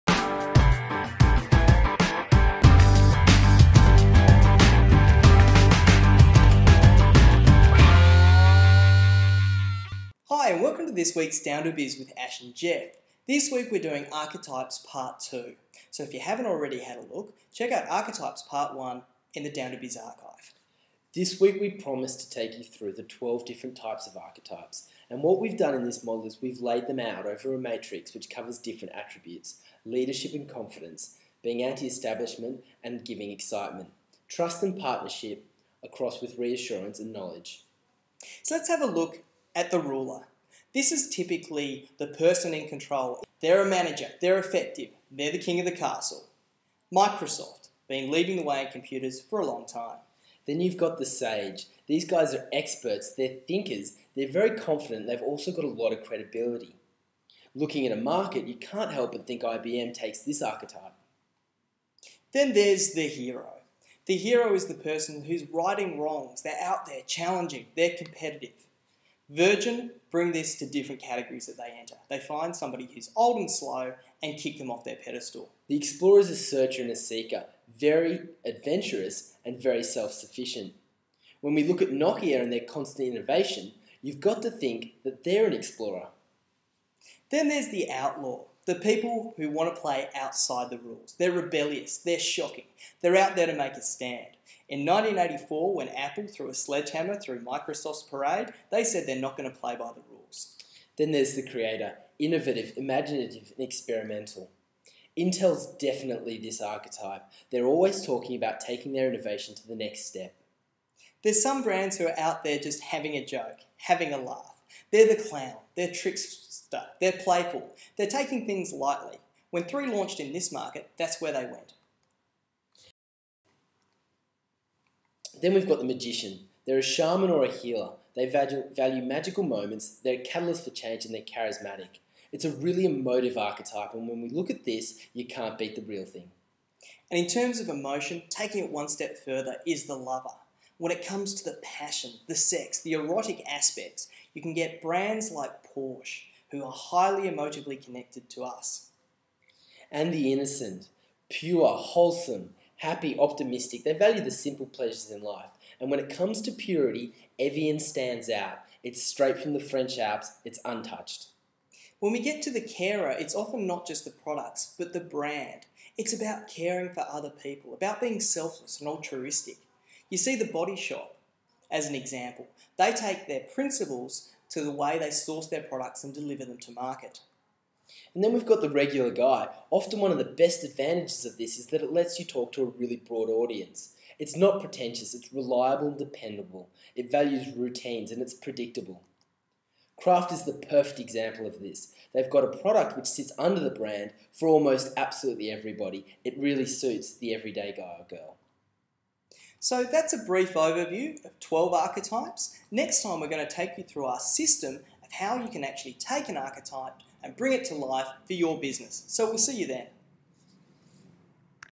Archetypes Part 2 - DOWNtoBIS #17
This is the second installment of a three part episode where Ashton Bishop and Jeff Cooper explore archetypes and their application to marketing. In part 2 the guys explore the different types of archetypes and provide an example of each.